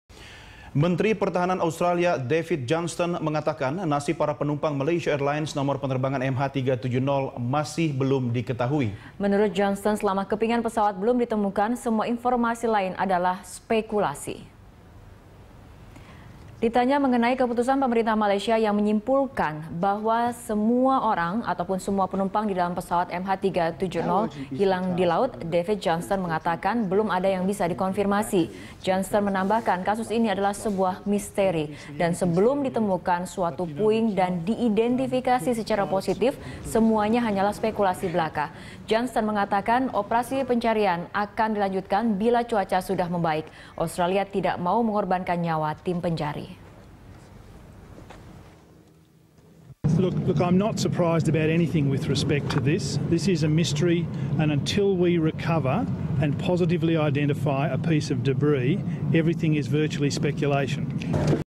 Menhan Australia: Keberadaan MH370 Masih Spekulasi
Menteri Pertahanan Australia David Johnston mengatakan nasib penumpang Malaysia Airlines masih belum diketahui. Menurut Johnston, selama kepingan pesawat belum ditemukan, semua informasi lain adalah spekulasi.

Facebook.com/BeritaSatuTV
Youtube.com/BeritaSatu